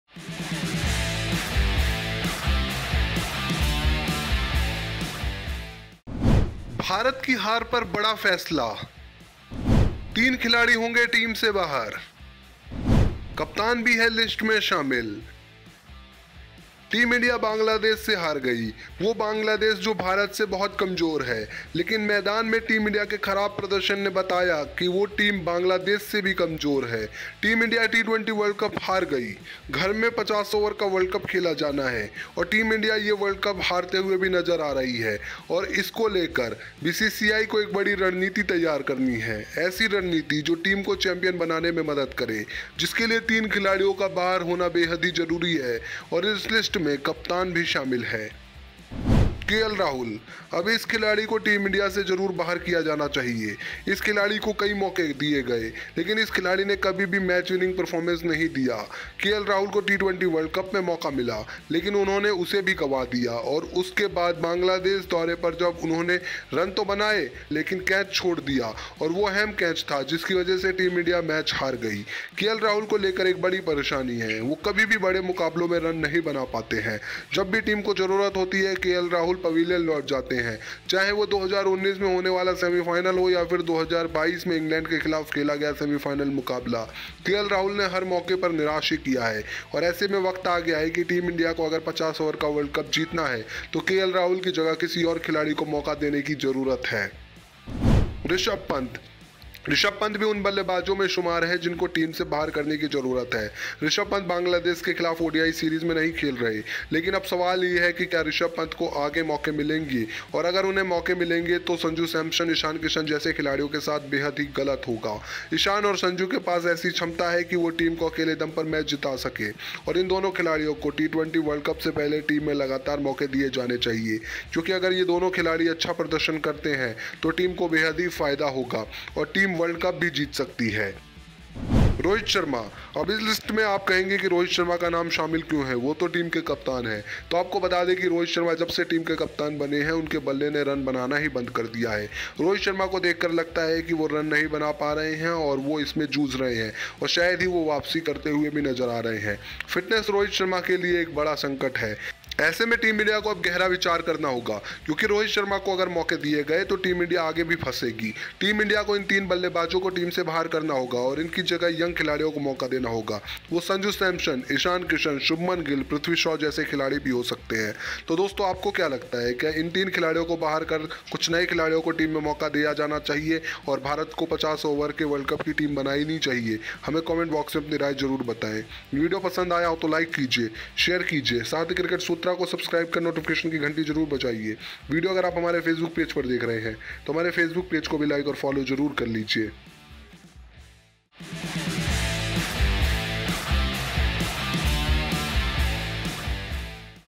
Team India की हार पर बड़ा फैसला, 3 खिलाड़ी होंगे बाहर, कप्तान भी शामिल | Rohit | BCCI
भारत की हार पर बड़ा फैसला
3 खिलाड़ी होंगे टीम से बाहर
कप्तान भी है लिस्ट में शामिल

* Cricket News
* Match Schedule
* Players Interviews
* Live Sessions
* IPL News
* BCCI News
* ICC Tournaments
* Depth analysis